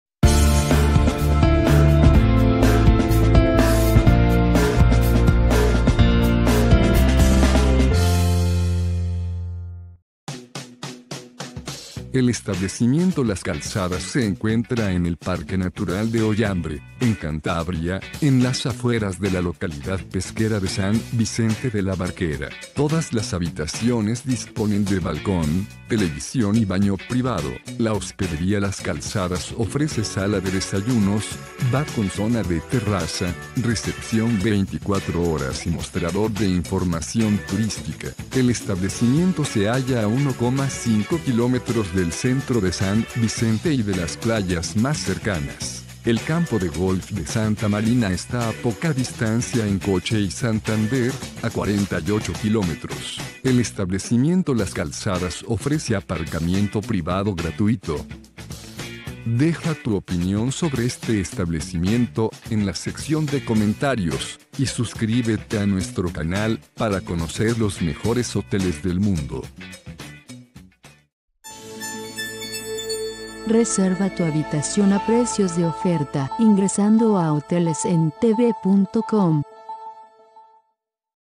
Hospedería Las Calzadas en San Vicente de la Barquera
Hospedería Las Calzadas goza de bar, recepción 24 horas y campo de golf (a menos de 3 km), además el edifico de Hospedería Las Calzadas se halla perfectamente ubicado en una excelente zona de San Vicente de la Barquera. El equipamiento de las habitaciones de Hospedería Las Calzadas se compone de bañera, tv y teléfono.